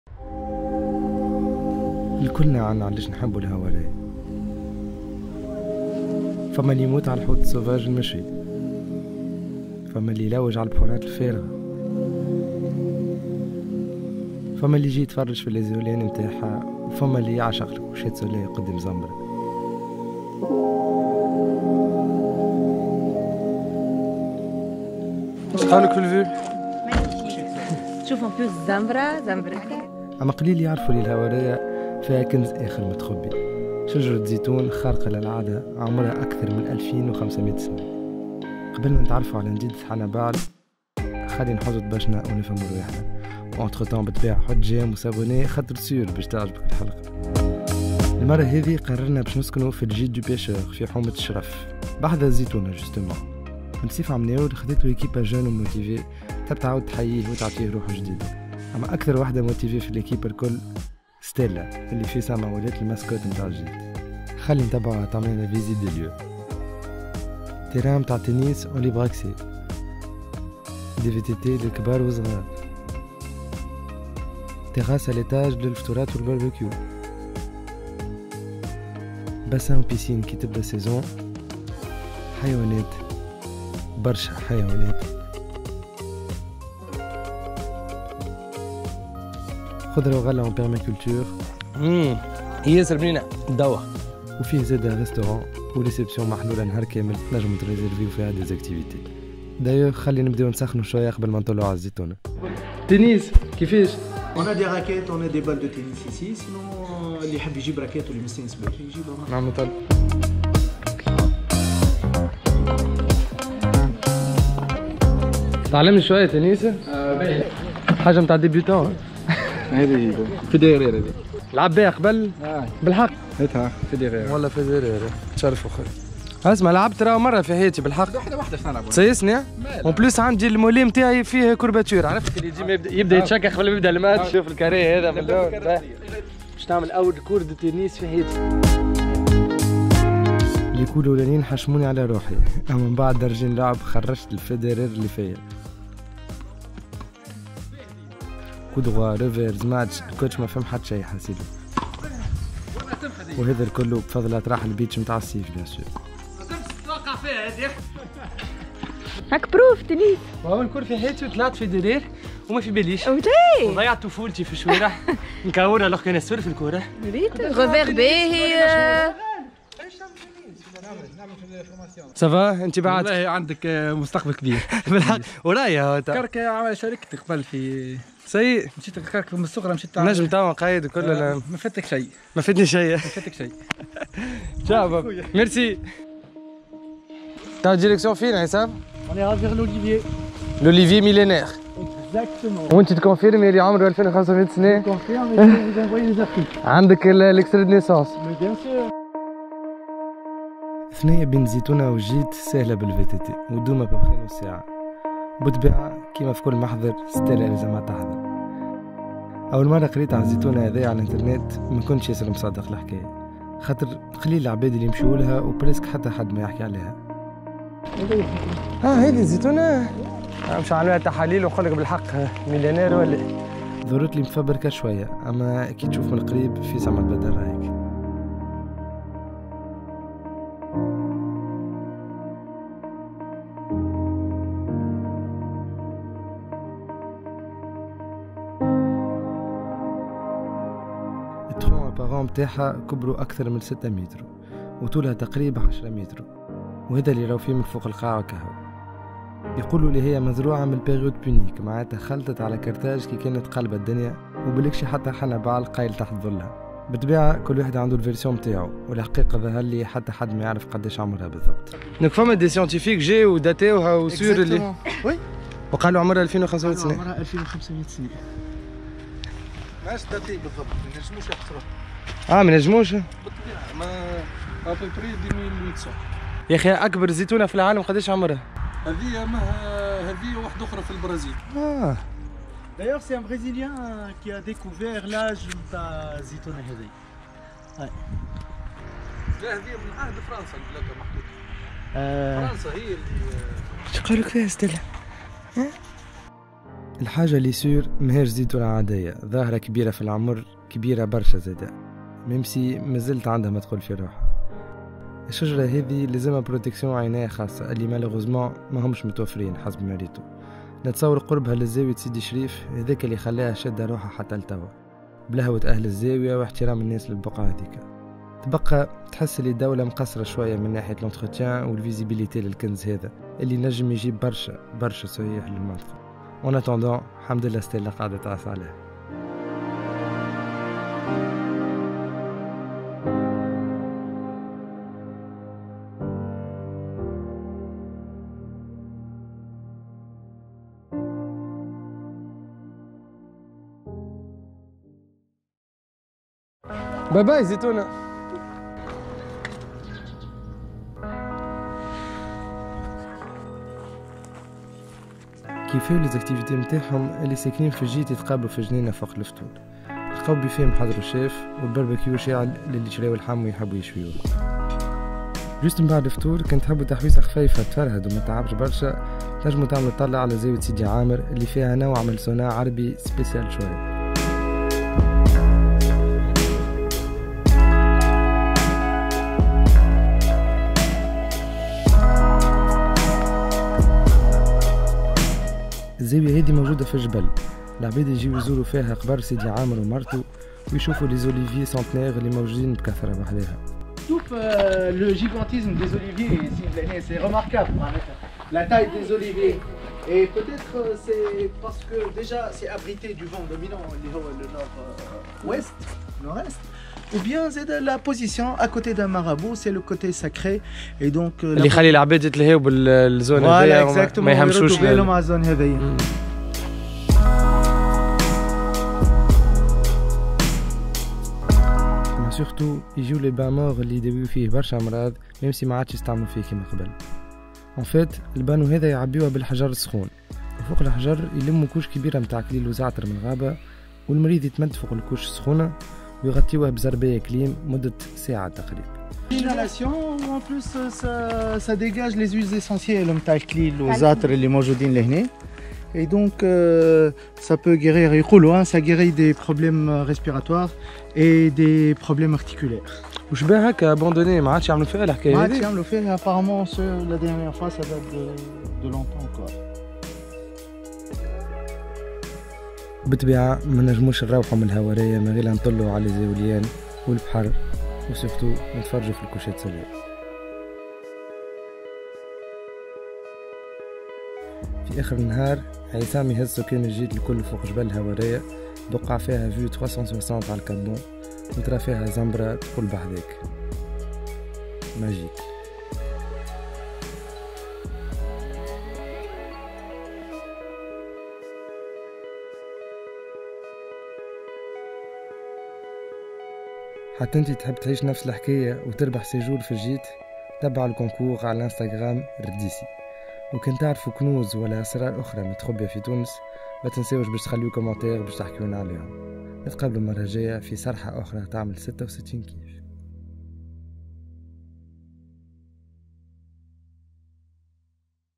On a visité le trésor oublié de Haouaria! كنز مخفي في الهوارية
Tu as déjà vu cet olivier légendaire à Haouaria? 😍

Du haut de ses branches, 2500 ans d'histoire nous contemplent 🌿 On dit qu'il a été planté durant la période punique, et qu'Hannibal aurait peut-être fait une sieste sous son ombre 😂

Dans ce nouvel épisode de 66KIF, on a rendu visite à ce petit trésor qui manque malheureusement d'entretien et de visibilité, alors qu'il pourrait ramener des milliers de visiteurs à la région 🙏